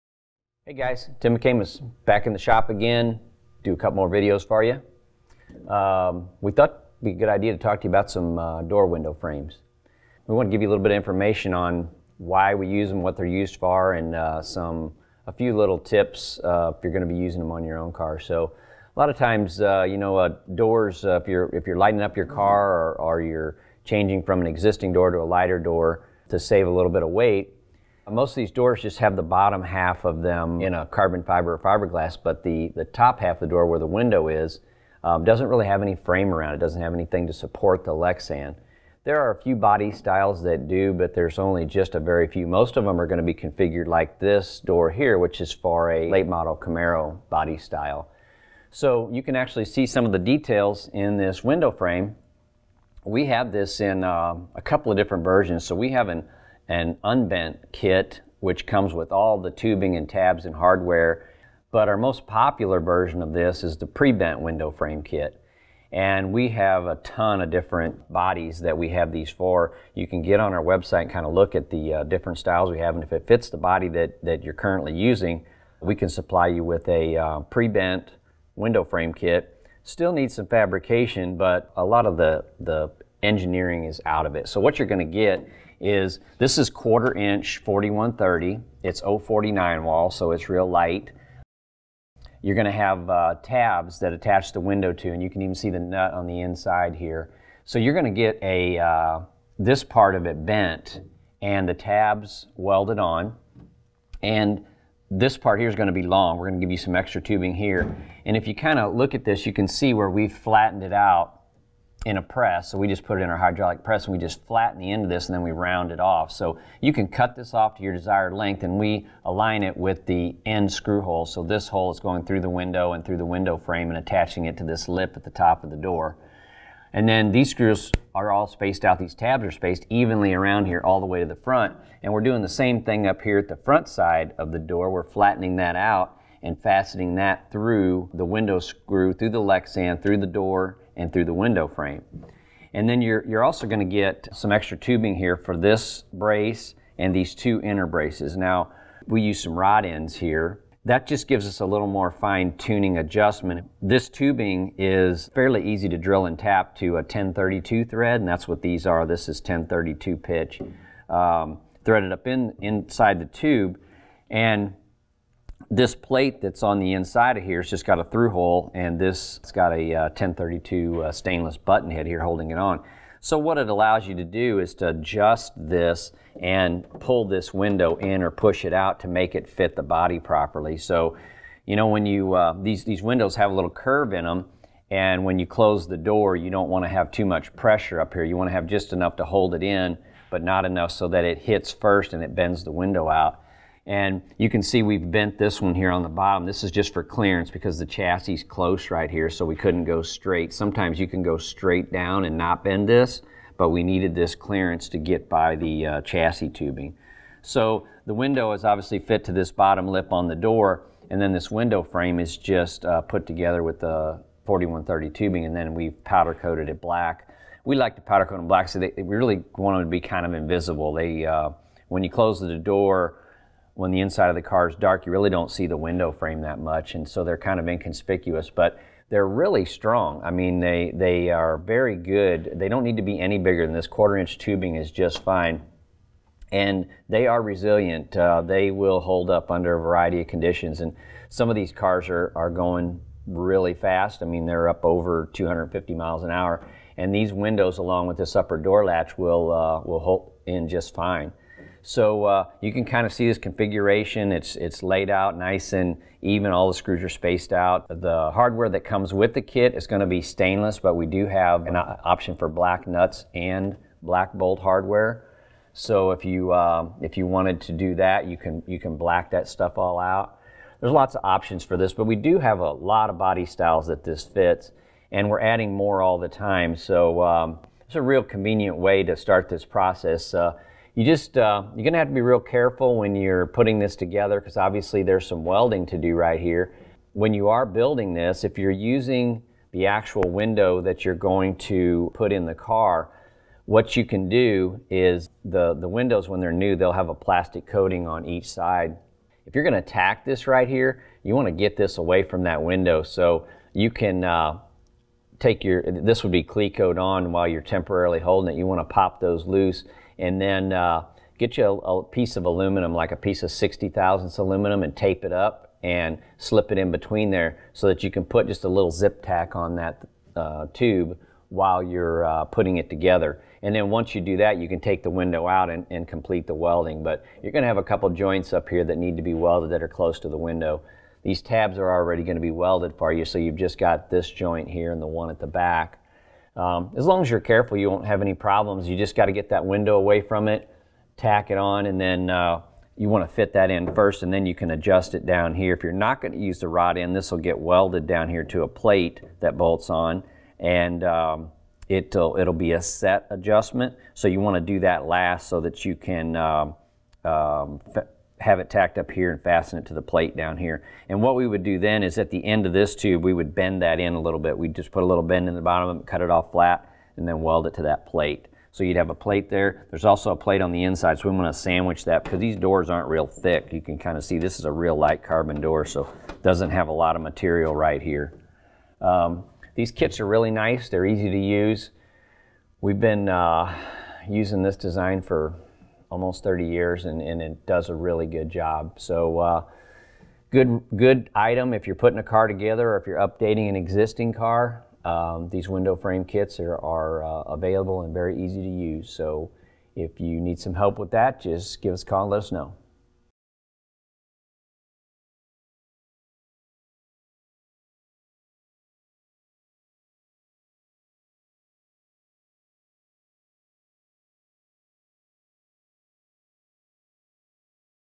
Door Window Frames
If you have any kind of fiberglass or carbon fiber door on your race car, chances are you have lexan or polycarbonate windows installed. Most doors are made without any type of outer support for the window.  To add strength and durability, door window frames are commonly installed.

Tim gives a quick breakdown in tonight’s video on the subject.